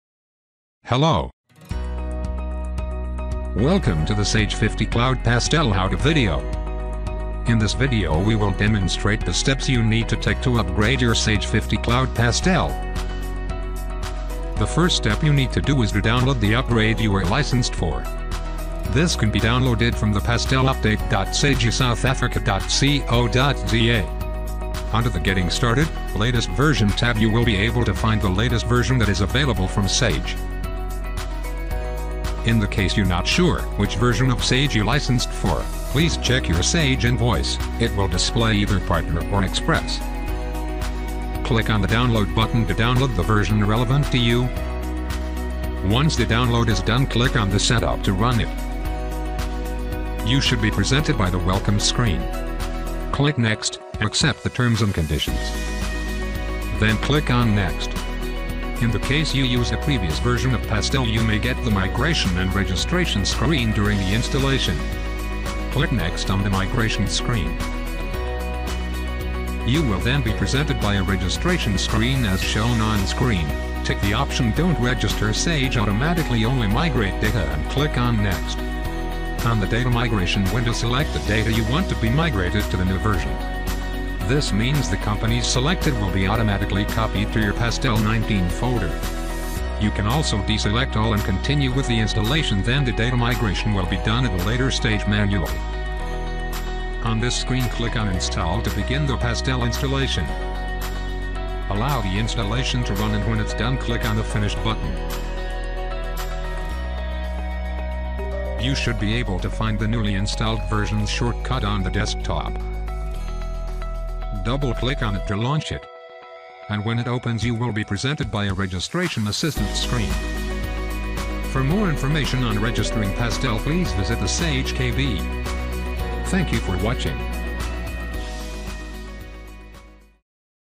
Sage 50cloud Pastel (ZA) - How do I upgrade to the latest version of Sage?
In this video you will learn how to upgrade to the latest version of Sage 50cloud Pastel Accounting.